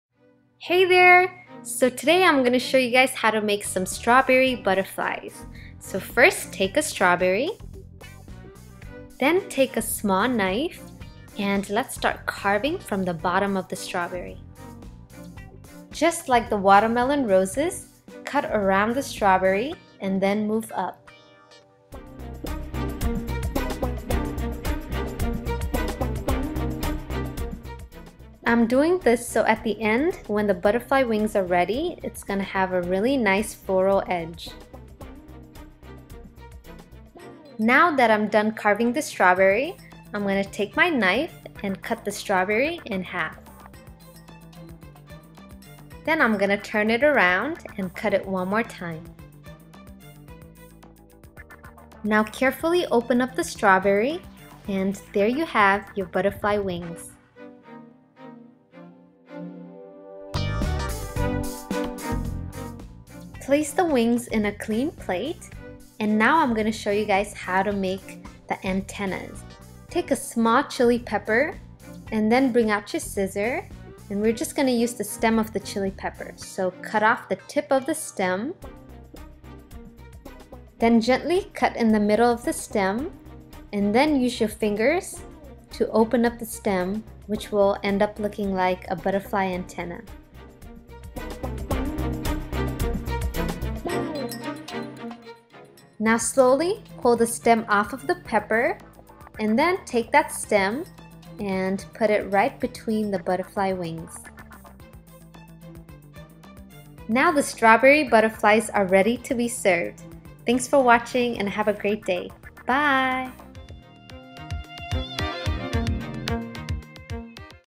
How to make Strawberry Butterflies !!! (Promise Phan )
Thanks for the great feedback on my last Watermelon Rose Tutorial. Inspired to do more food art !
If You guys decide to try this out ..Make sure to Share your results with me

You'll need
- Strawberries
- Cutting board
- knife
- chili pepper
- Plate